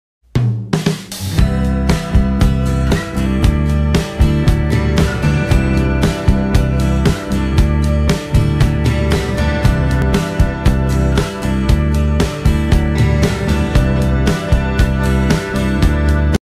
Space truss airport building
Large Span Steel Structure Space Frame Truss Roof Design Airport Terminal Building Construction,Large space, beautiful appearance, easy to install.